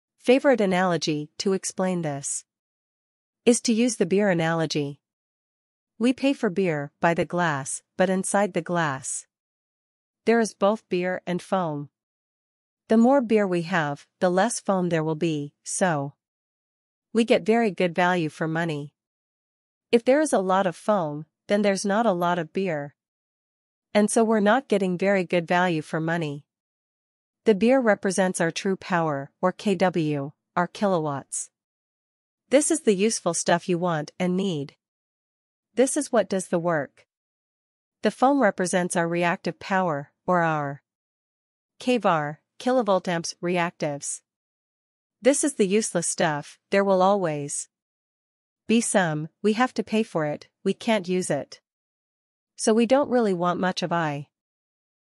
Analogy of power factor wiy glass of beer
Favourite analogy to explain this
is to use the beer analogy.
We pay for beer by the glass, but inside the glass
there is both beer and foam.
The more beer we have, the less foam there will be, so
we get very good value for money.
If there is a lot of foam, then there's not a lot of beer,
and so we're not getting very good value for money.
The beer represents our true power or kW, our kilowatts.
This is the useful stuff you want and need.
This is what does the work.
The foam represents our reactive power, or our
kVar, Kilovolt-Amps Reactives.
This is the useless stuff, there will always
be some, we have to pay for it, we can't use it
so we don't really want much of i